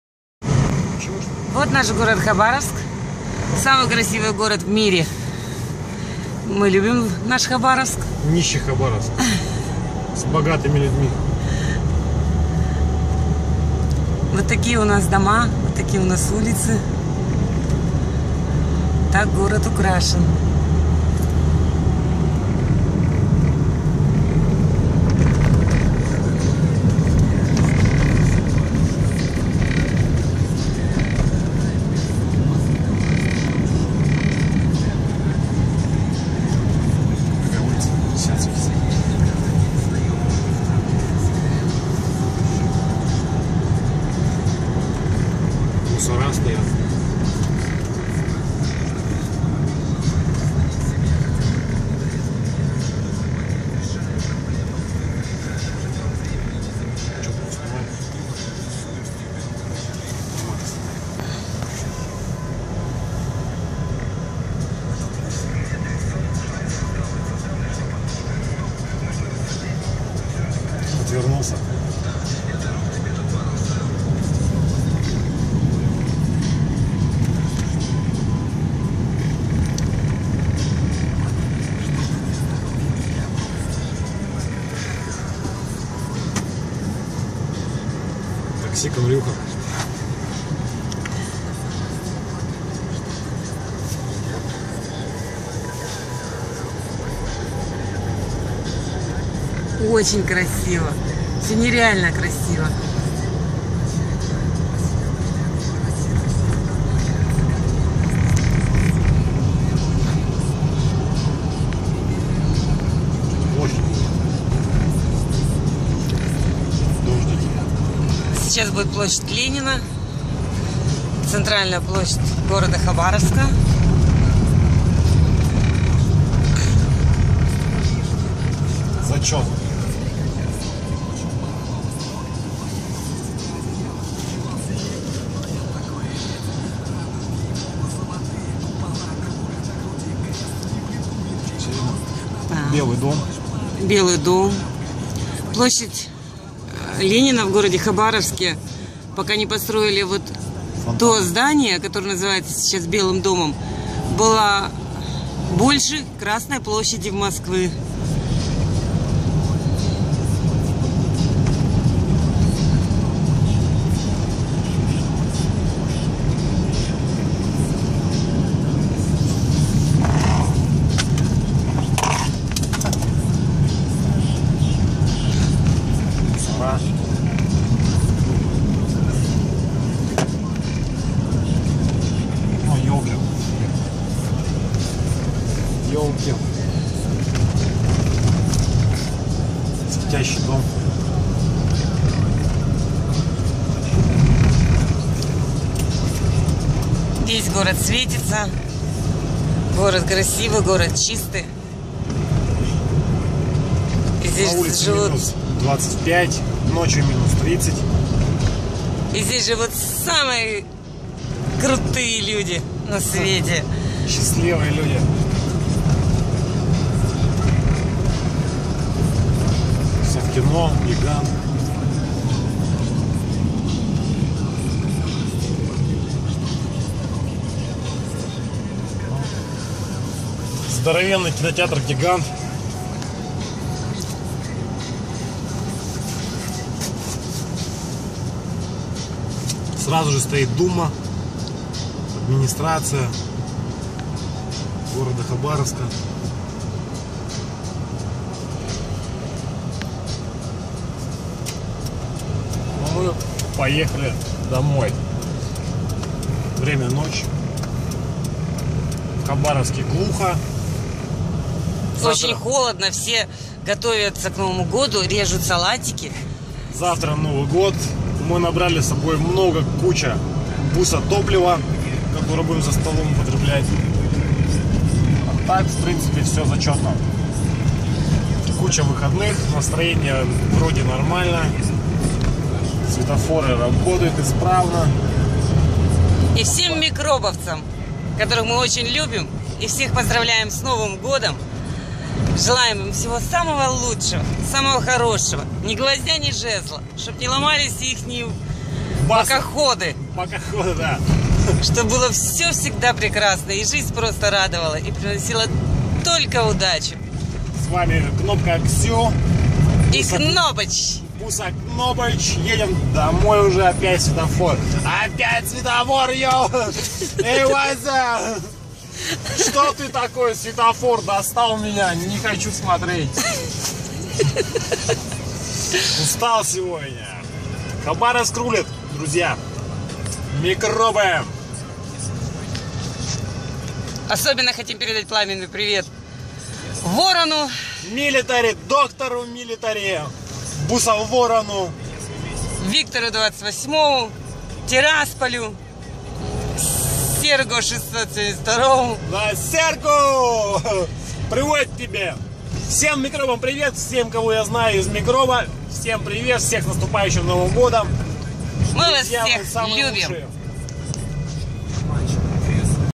Новогодний Хабаровск 30 10 2013
пред Новогодняя ночь в Хабаровске ул Карла Маркса
посвещается клубу Микроб . ру